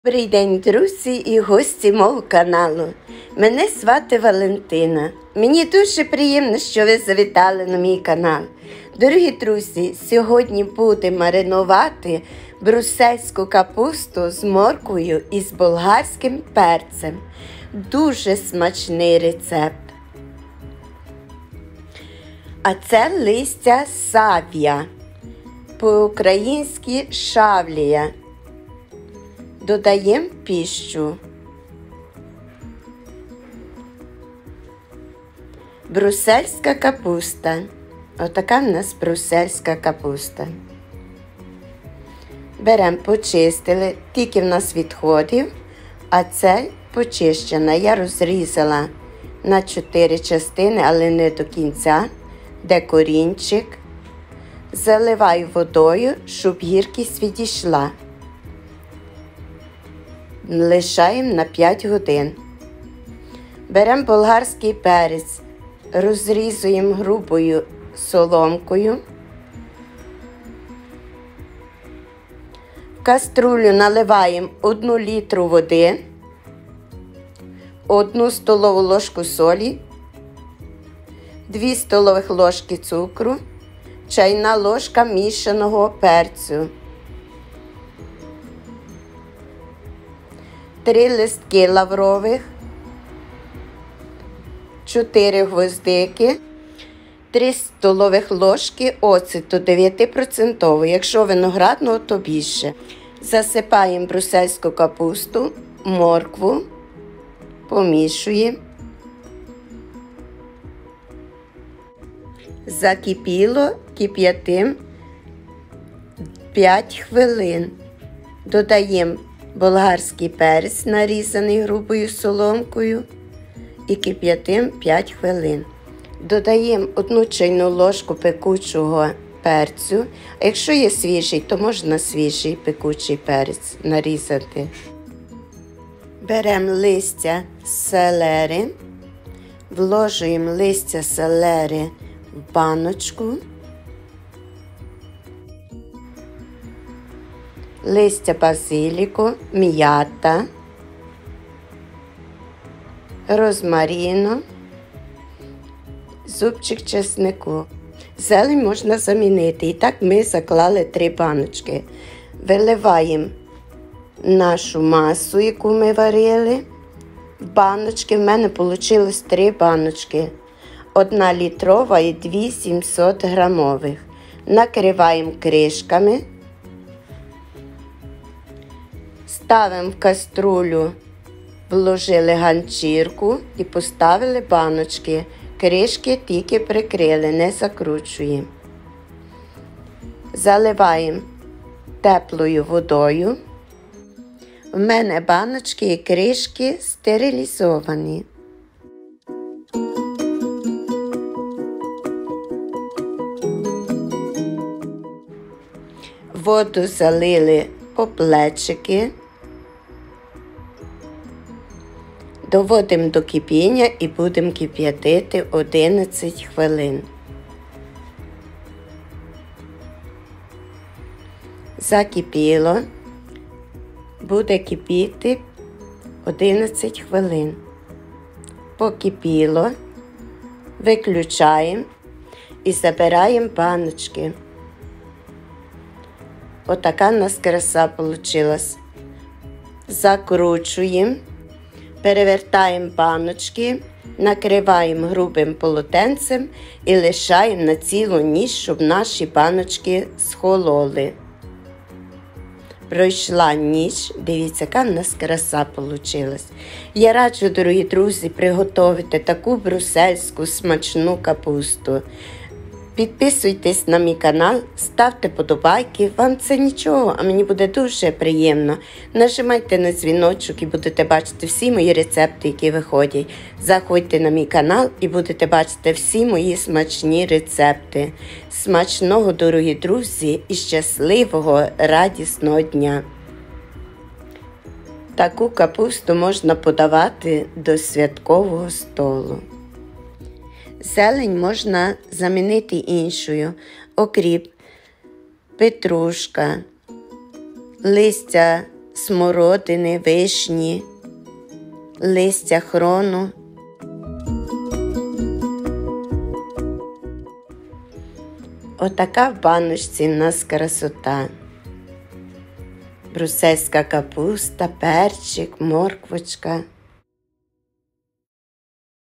МАРИНОВАНА БРЮСЕЛЬСЬКА КАПУСТА : ВІТАМІНИ на ЗИМУ
Інгредієнти:

Маринад:
1 літр води
1 ст. ложка солі
2 ст. ложки цукру
1 ч. ложка різних видів перцю горошком
3 листа лаврових
4 гвоздики
3 ст. ложки оцту 9-%
1 ч. ложка пекучого сухого перцю або одна свіжа перчина, порізана шматочками

1 кг брюсельської капусти
1 перець болгарський
2 морквини
зелень селери
базилік
розмарин
м’ята
шавлія
3 зубчики часнику

Зелень можна брати будь-яку, за бажанням